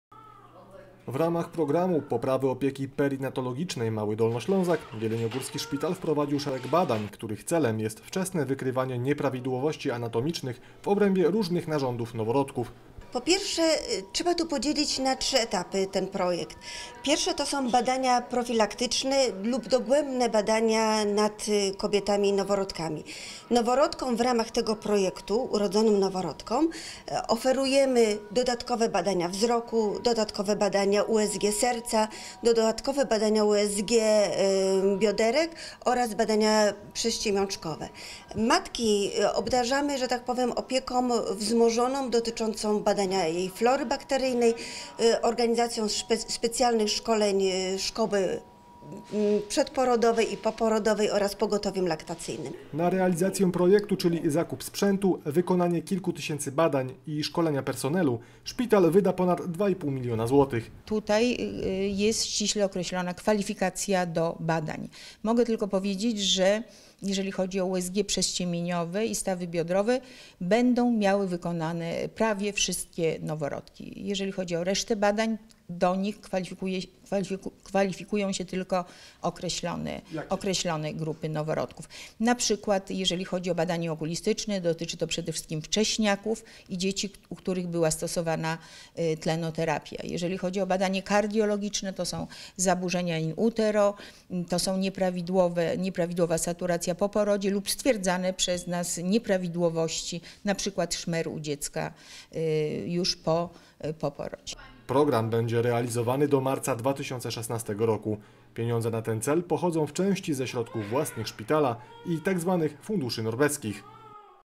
Jeleniogórski szpital wprowadził kompleksowe badania noworodków
Kompleksowa diagnostyka noworodków pod względem wykrywania wszelkich dysfunkcji. Wojewódzkie Centrum Szpitalne Kotliny Jeleniogórskiej wprowadziło szereg badań, którym poddawane są urodzone tu dzieci.